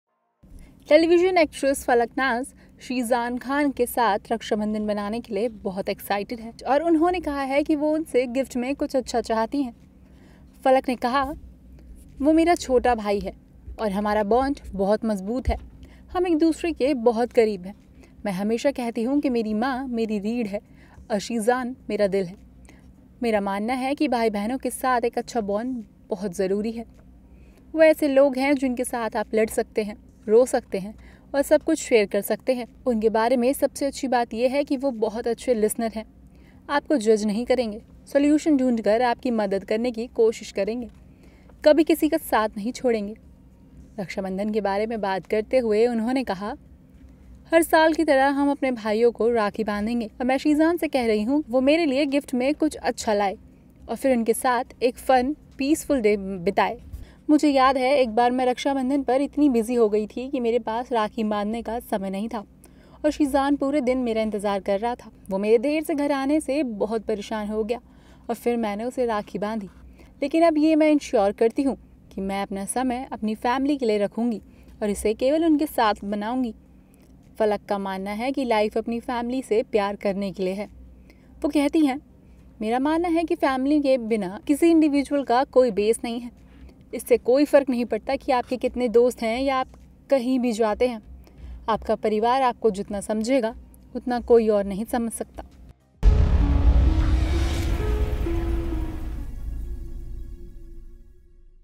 Rakshabandhan plans पर Falaq Naaz : Sheezan से कह रही थी कि मेरे लिए gift के रूप में कुछ अच्छा लाओ’
टेलीविजन अभिनेत्री फलक नाज़ शीज़ान खान के साथ रक्षाबंधन मनाने के लिए बहुत उत्साहित हैं और उन्होंने कहा कि वह उनसे उपहार के रूप में कुछ अच्छा चाहती हैं।

Teevision actress Falaq Naaz is too excited to celebrate Rakshabandhan with Sheezan Khan and said that she wants something good as a gift from him.